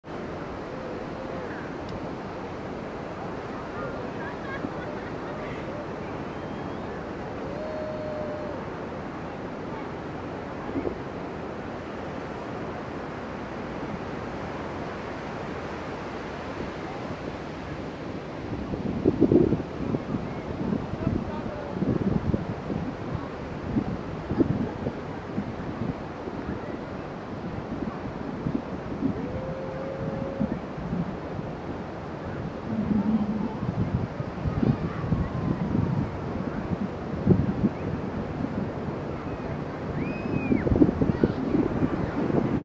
Gidget Surfs Big Long Wave
Surf Gidget The Pug Rode the Biggest & Longest Wave So Far For A !5lb Pug. This small but mighty pug surfs hard, does 360's & even surfs backwards.

Filmed: GOPRO Hero Sessions & Hero 6
Soloshot